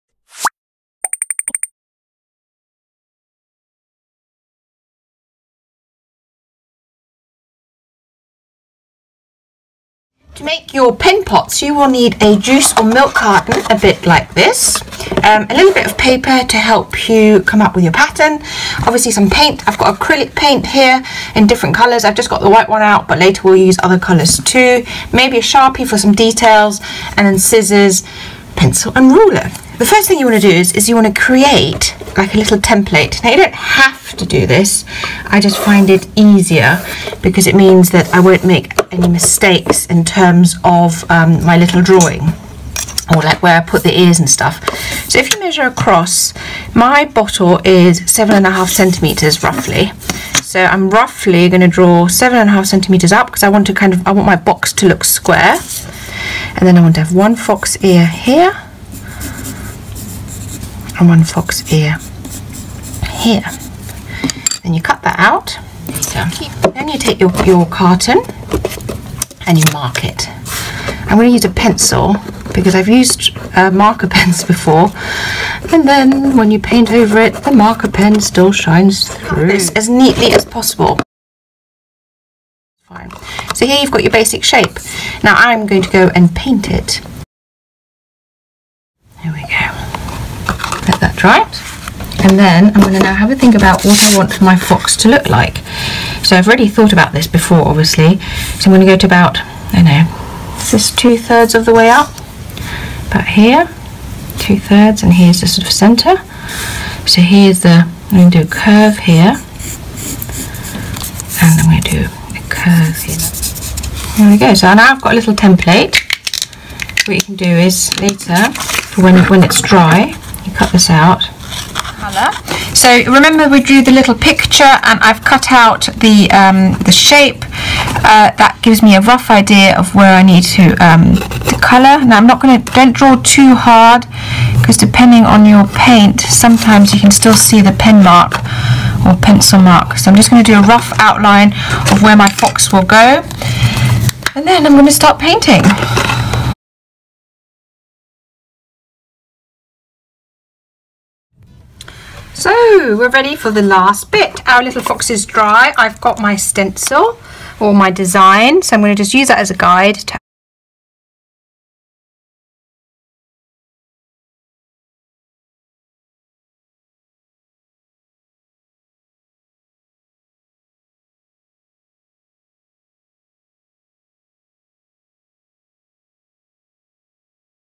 Fox Pen Pot - upcycled juice carton Desk Tidies (BONUS VIDEO)
BONUS VIDEO - I have shared this Fox Pen Pot before - but this is a "quick view" of the project that I thought you may enjoy. Leanr how to design your own animal pen pots and get making! CLICK FOR INFO